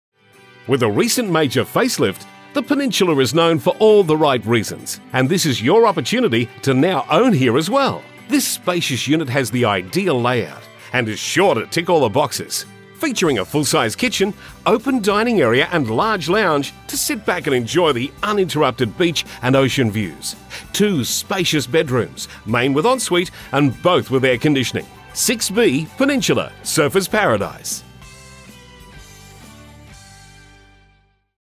6B Peninsula 5 Clifford Street Surfers Paradise 4217 QLD by ...
With a recent major face-lift The Peninsular is known for all the right reasons and this is your opportunity to now own here as well. This spacious unit has the ideal layout and is sure to tick all the boxes. Featuring a full size kitchen, open dining area and large lounge to sit back and enjoy the uninterrupted beach and ocean views, 2 spacious bedrooms, main with ensuite and both with air-conditioning.6b Peninsular, 5 Clifford Street, Surfers Paradise